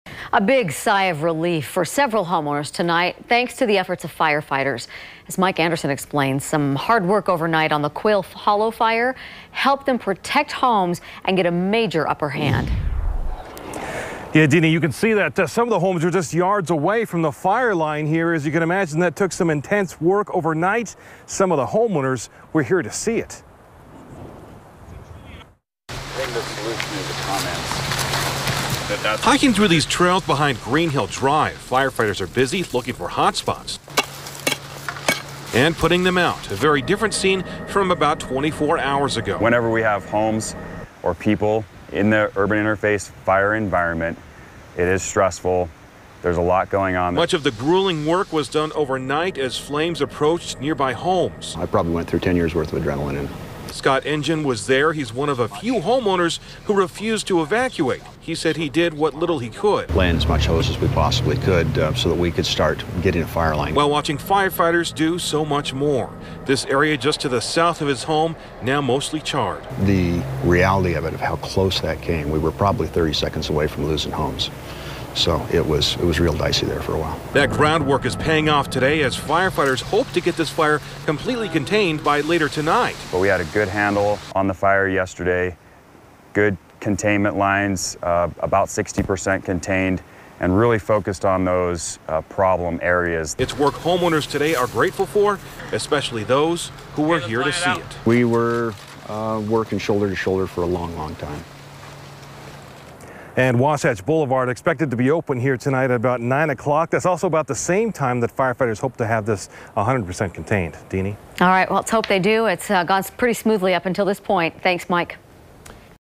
Quail Hollow Fire 100 percent contained, officials say
Fire crews achieved 100 percent containment of the Quail Hollow Fire by 7 p.m. Wednesday.

news,traffic,weather,sports,classifieds,cars,jobs,homes,television,radio,salt lake,utah,local